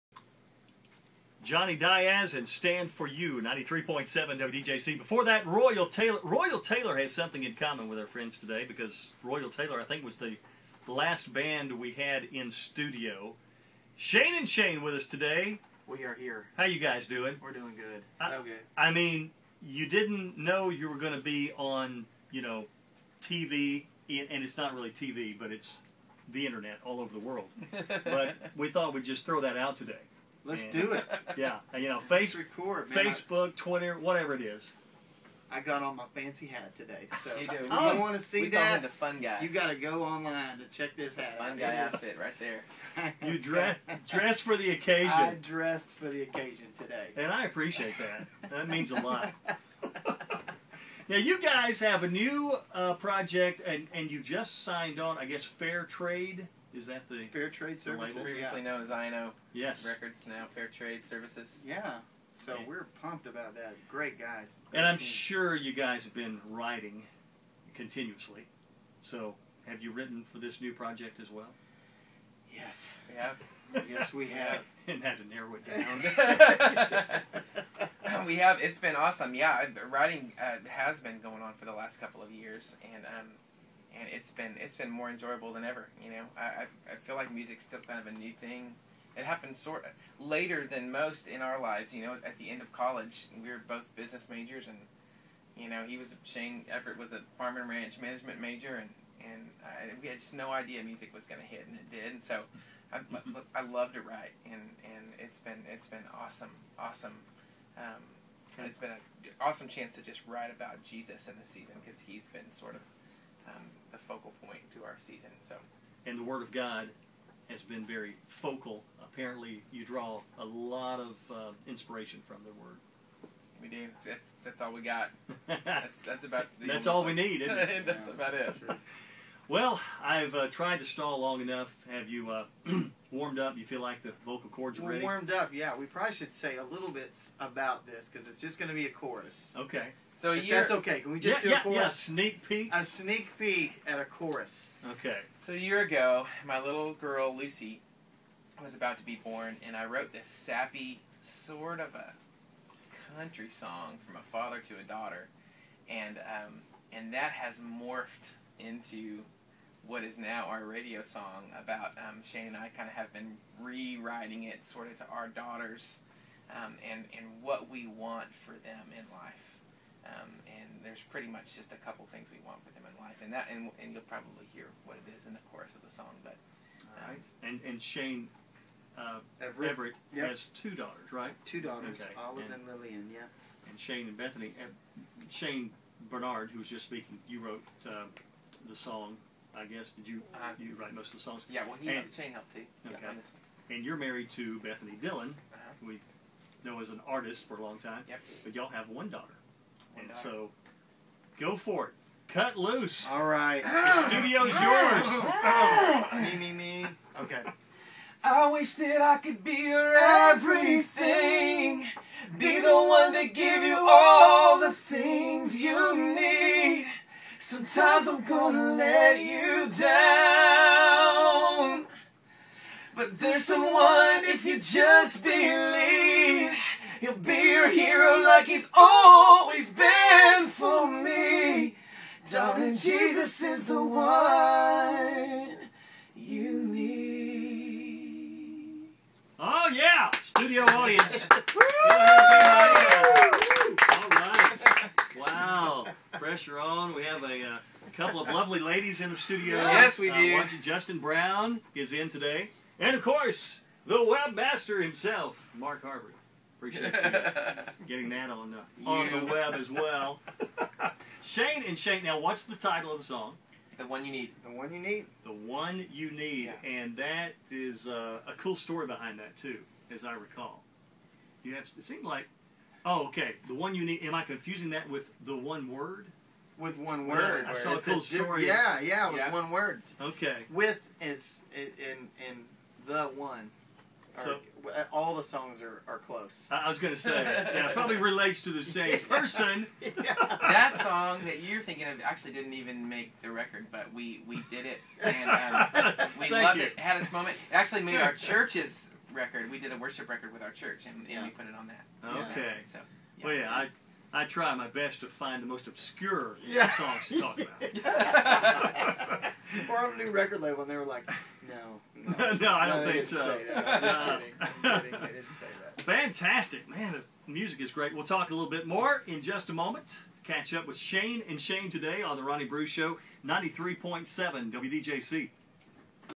Shane and Shane with Ronnie Bruce
Shane and Shane stopped by to talk to Ronnie Bruce about music, writing and their show that night.  They even sang for us!